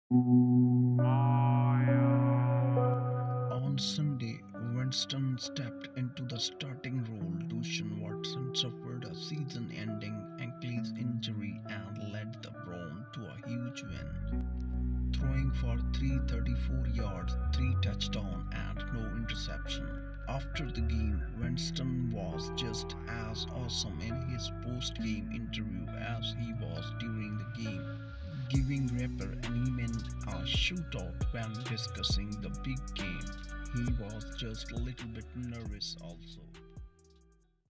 Jameis Winston's Postgame Interview After Win vs Ravens, Gives Eminem A Shoutout
Jameis Winston Gives Eminem A Shoutout During Postgame Interview After Win vs Ravens